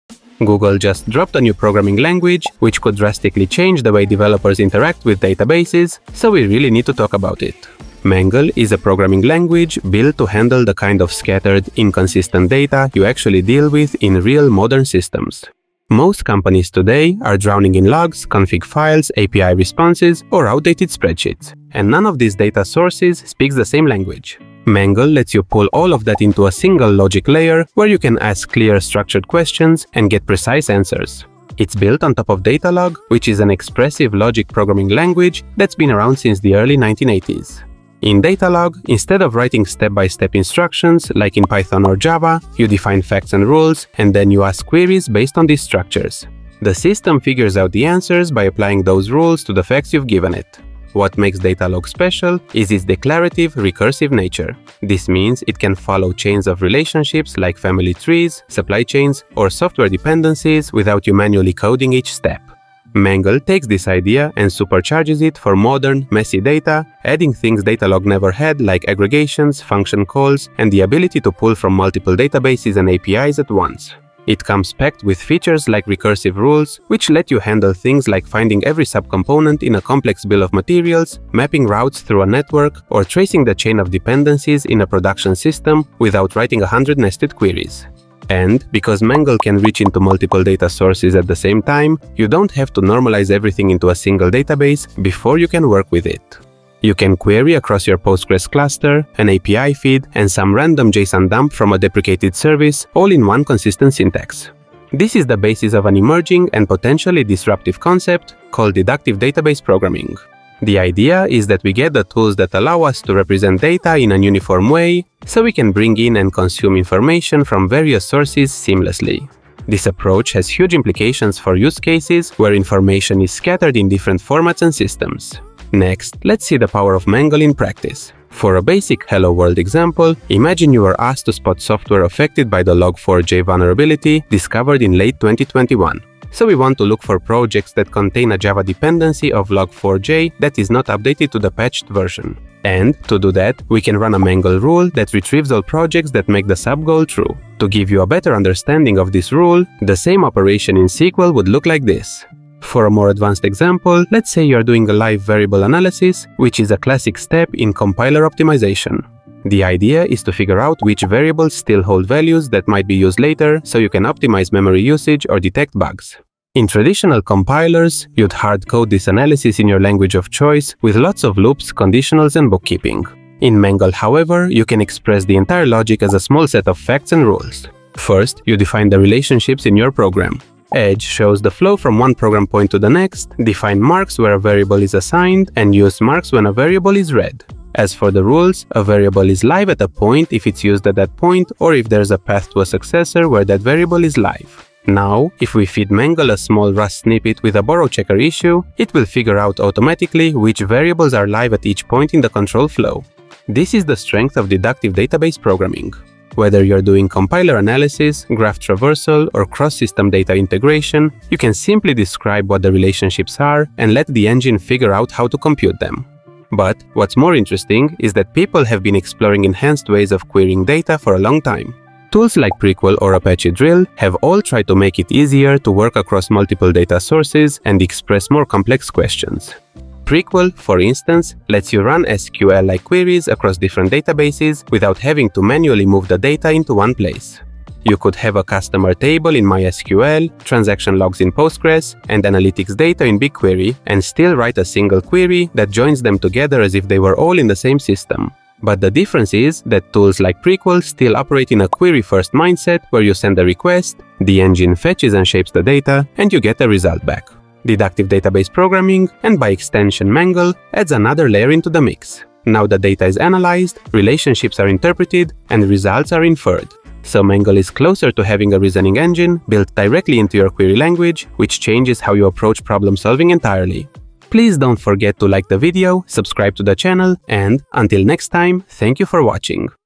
Google just dropped a new language for deductive database programming...
A quick look at the Google Mangle language.

💬 Topics:
- Google’s new Mangle language;
- Built on Datalog principles;
- Handling messy, scattered data;
- Deductive database programming;
- Queries across multiple sources;
- Built-in reasoning engine.